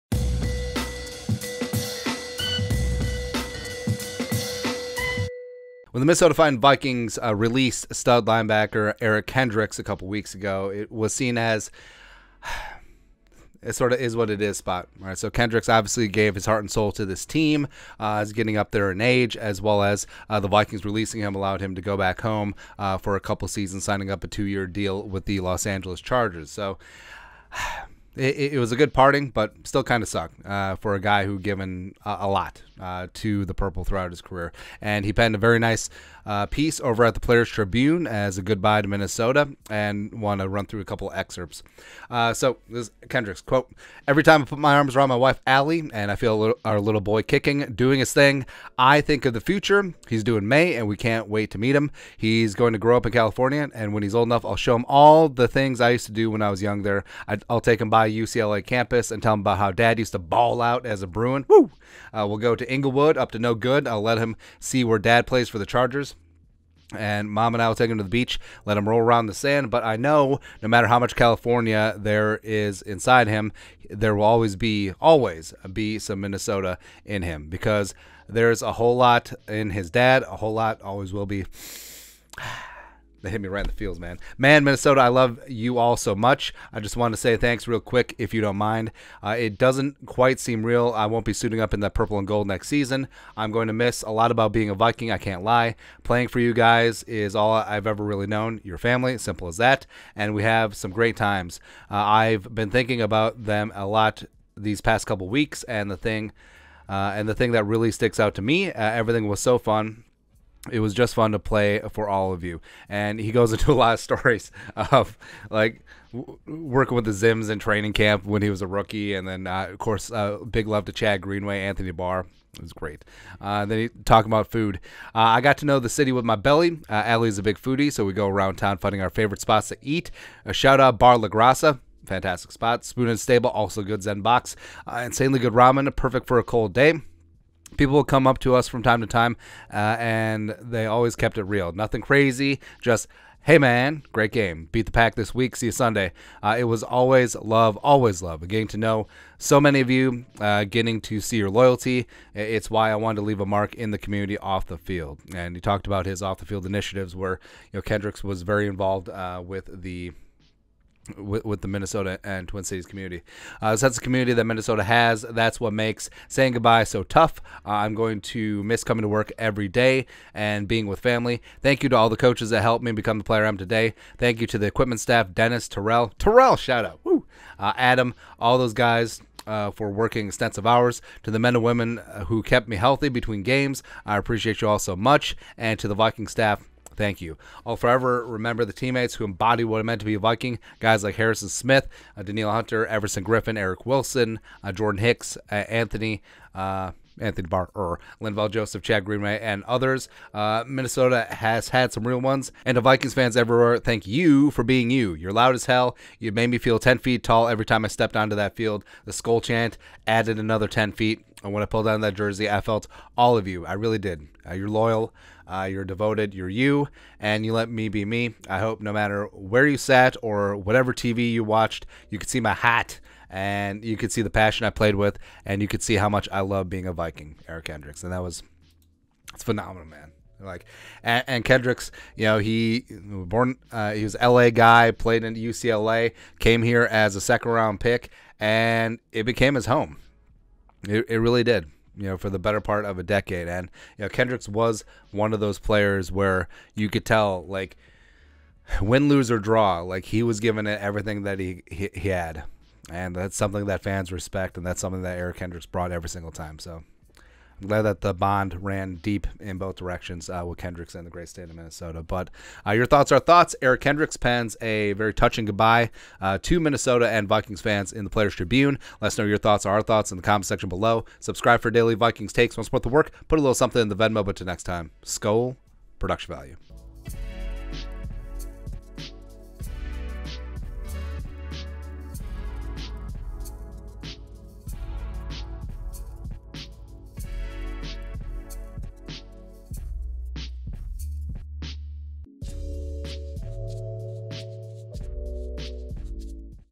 Eric Kendricks Pens Beautiful Goodbye to Minnesota in The Player's Tribune
Former Minnesota Vikings star linebacker Eric Kendricks penned a heartfelt goodbye to fans and the State of Minnesota in The Player's Tribune. Kendricks was released earlier in the month and signed a 2-year deal with his hometown Los Angeles Chargers.